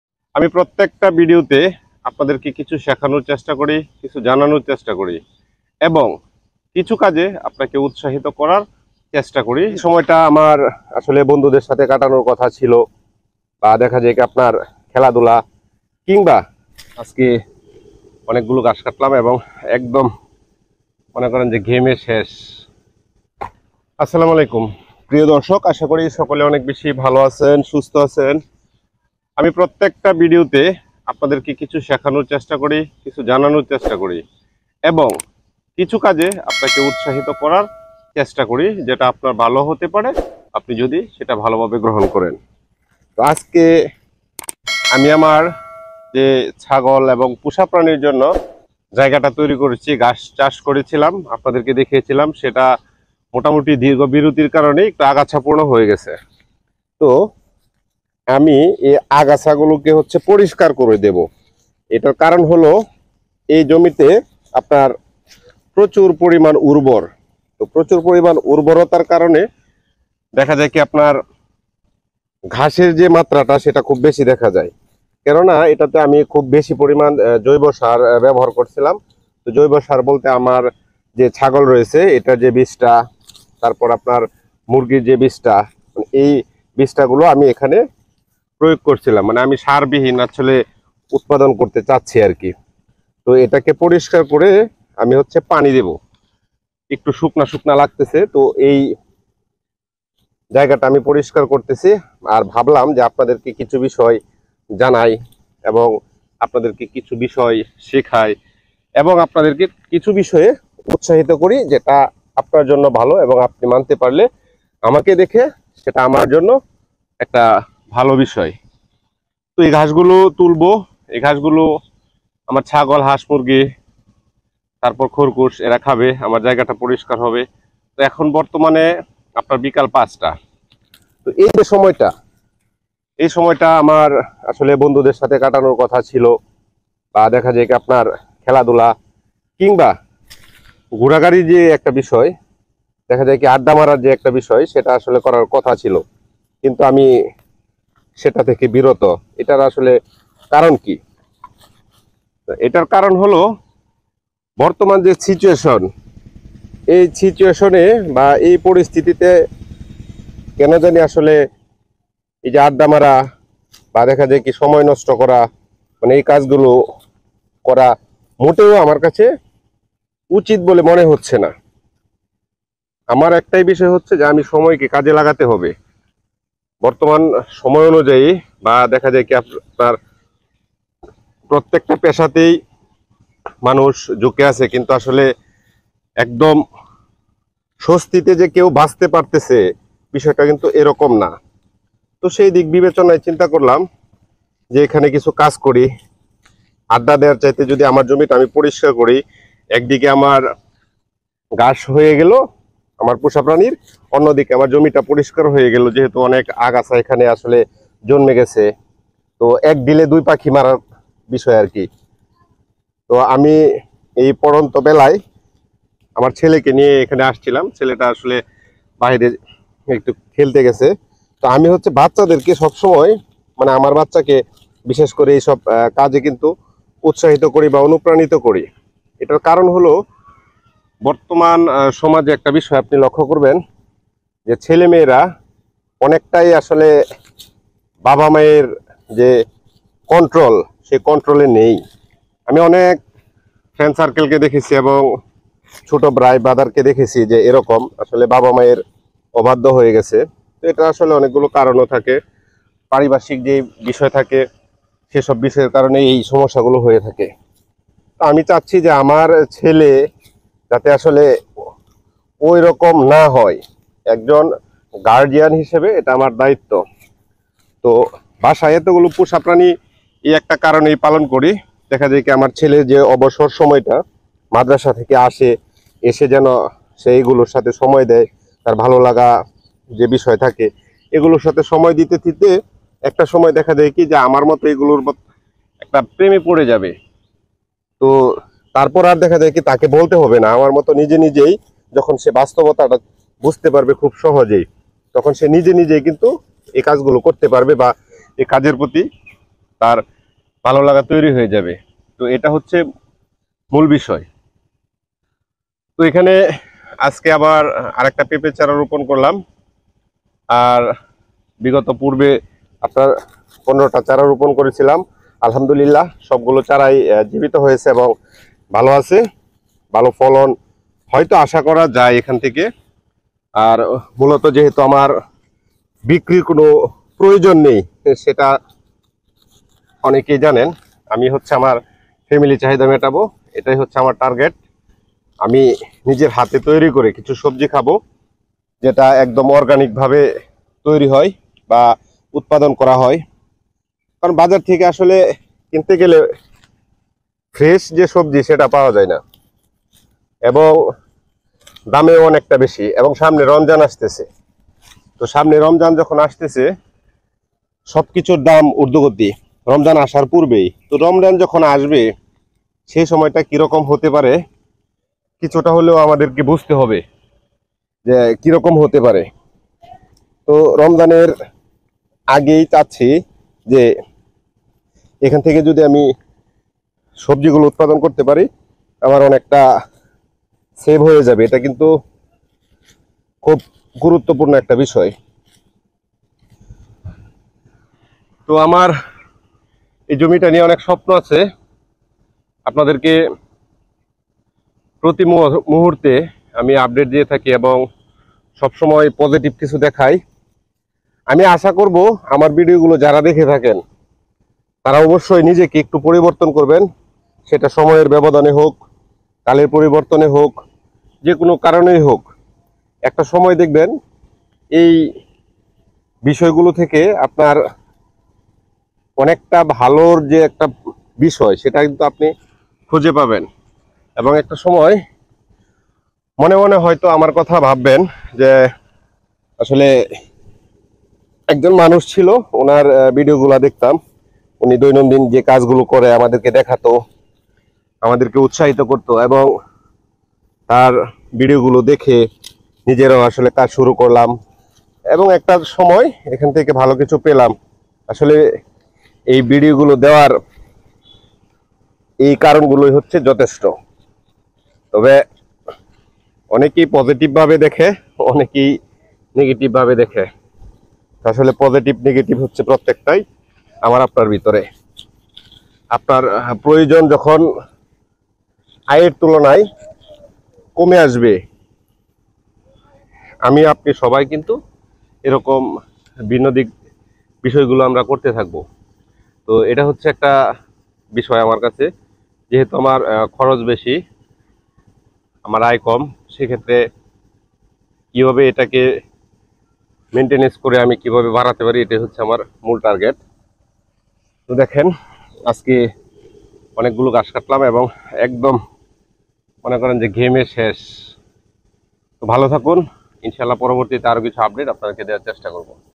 ঘাসের জমি থেকে অনেক কিছু পেয়েছি- নূরের পোষা প্রাণী
আসসালামু আলাইকুম আমাদের "নূরের পোষা প্রাণী" ইউটিউব আপনাদের সবাই কে স্বাগতম

আজকের ভিডিও তে তুলে দরবো আমার ঘাসের জমিতে কেন আমি এত সময় দিচ্ছি।

এই সময়ের সাথে সাথে সন্তানদের ও কিছু শিক্ষা দেয়া যায় খুব সহজে।

যা পরিবার সন্তান কিংবা নিজের জন্য খুব জরুরি বর্তমান সময় অনুযায়ী।

আমাদের এই চ্যানেলে প্রতিনিয়ত পোষা প্রাণী নিয়ে ব্লগ করে থাকি